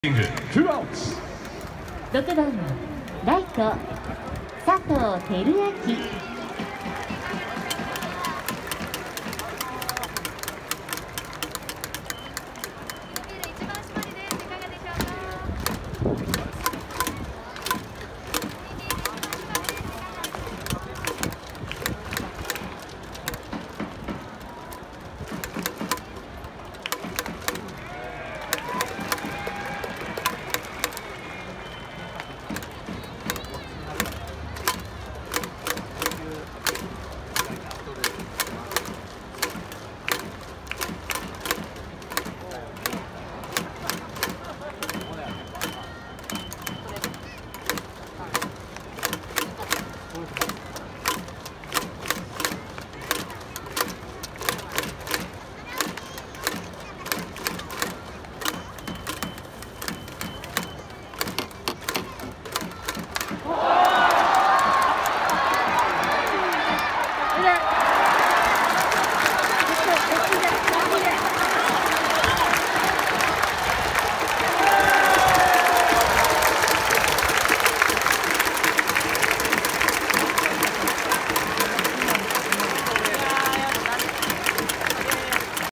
【20220816】公式戦 ヤクルト - 阪神 明治神宮野球場【4回表 佐藤輝】
4回表阪神の攻撃
6番 佐藤 輝明 二死走者なし
フェンス直撃の三塁打を放つ 3塁

(S2指定席/最上段)